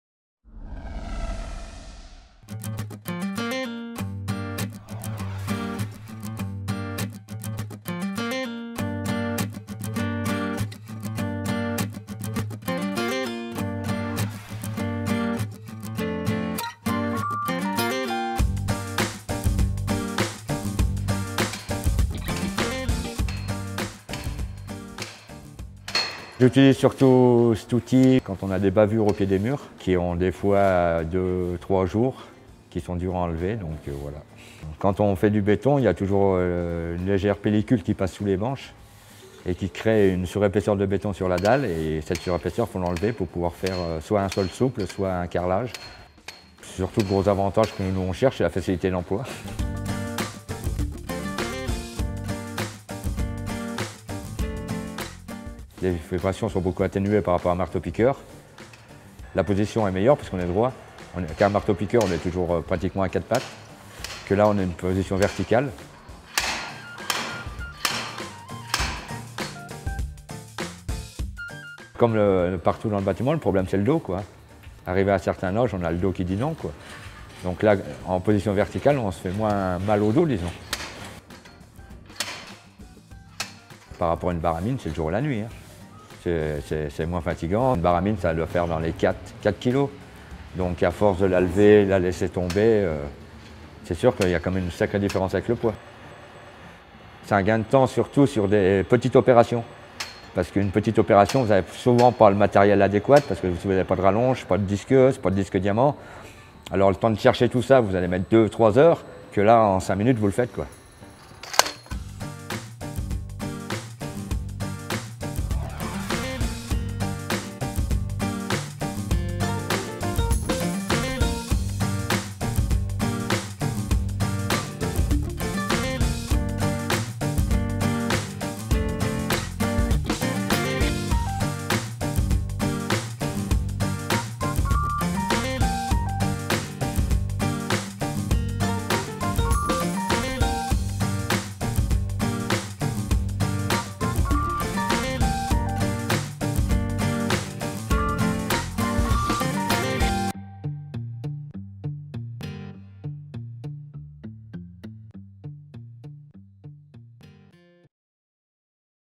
Présentation du burineur manuel nanovib Leborgne
30% plus léger et plus précis qu'une barre à mine traditionnelle